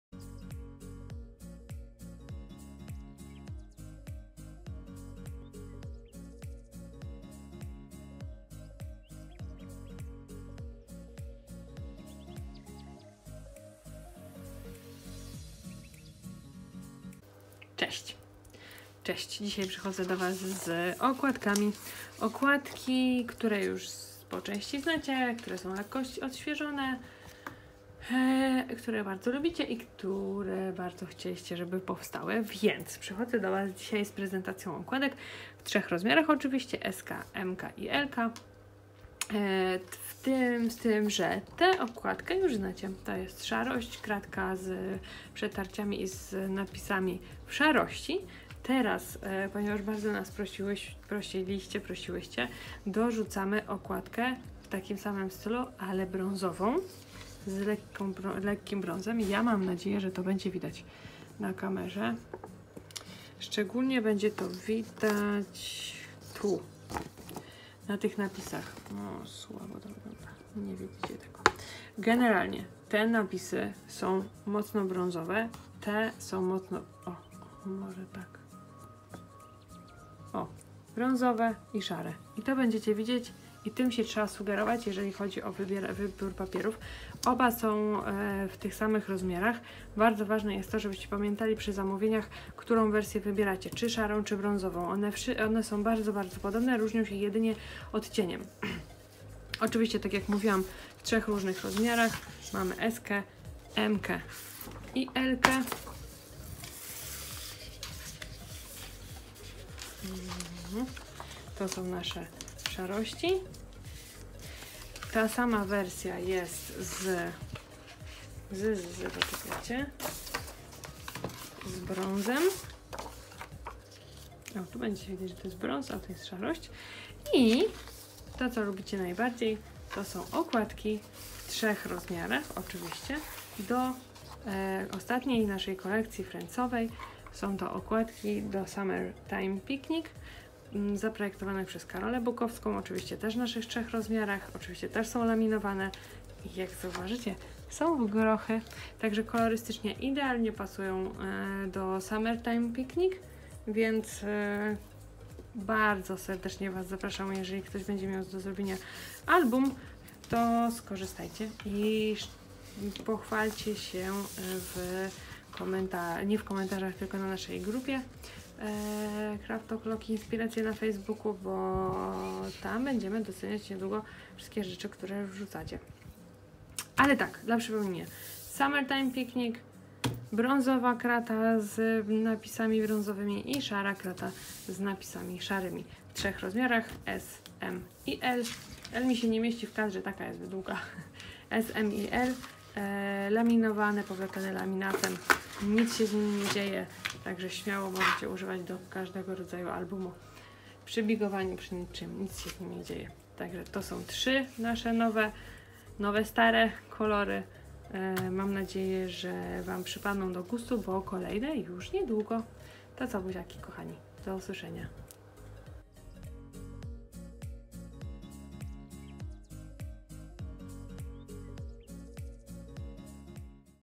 PREZENTACJA produktu - OKŁADKI laminowane
Kolejna propozycja okładek albumowych!
Tym razem w kolorach pasujących do Summertime picnic! Nie mogło również zabraknąć białych z przetarciami - i tu niespodzianka! Występują w dwóch wersjach! Z szarością i brązem!